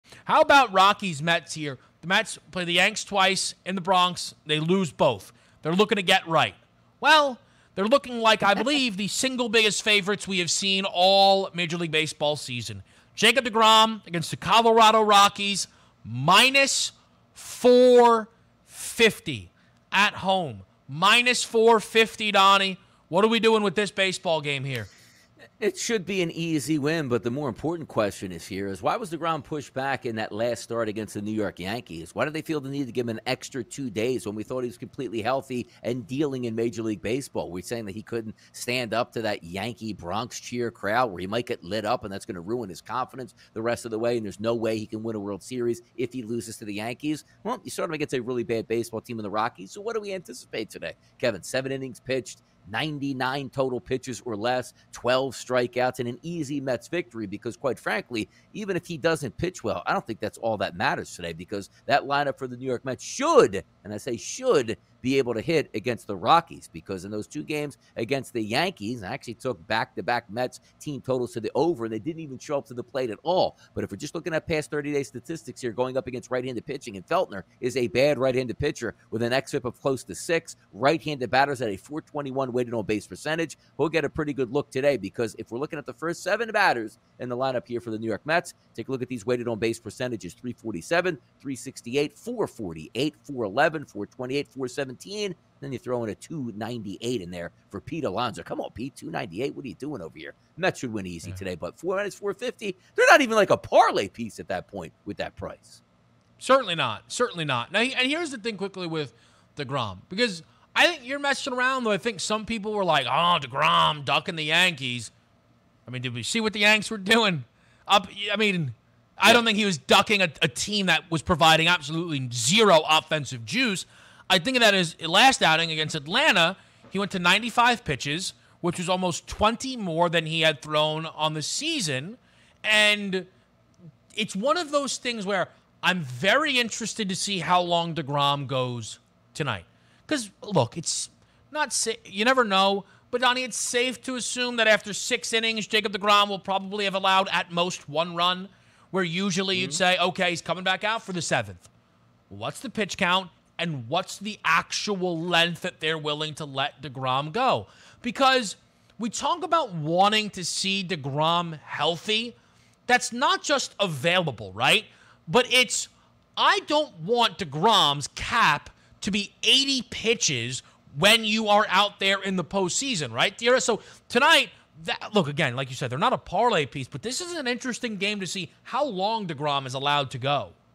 MLB 8/25 Preview: Rockies Vs. Mets
Kevin and Donnie preview the National League showdown between the Colorado Rockies and New York Mets on Thursday night in the MLB.